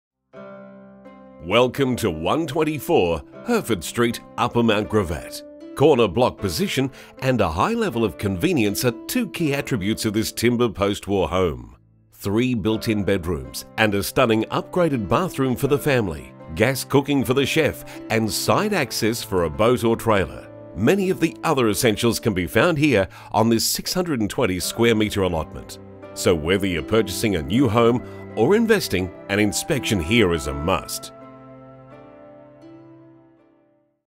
124 Hertford Street, Upper Mount Gravatt Queensland By Michael Foo
Property Video shoot of 124 Hertford Street, Upper Mount Gravatt Queensland by PlatinumHD for Brad Bell Real Estate